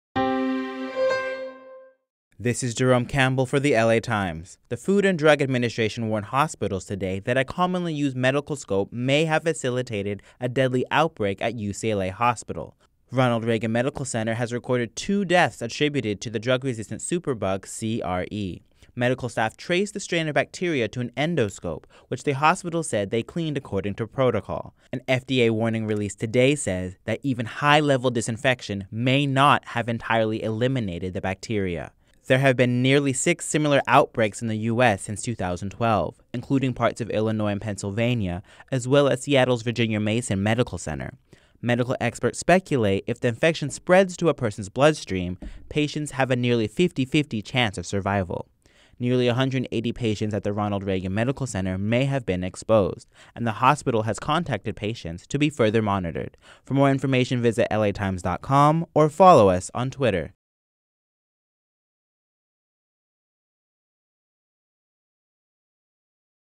FDA attributes spread of superbug to endoscope
Drug-resistant bacteria killed two patients at the UCLA medical center. The FDA says the bacteria passed through an endoscope.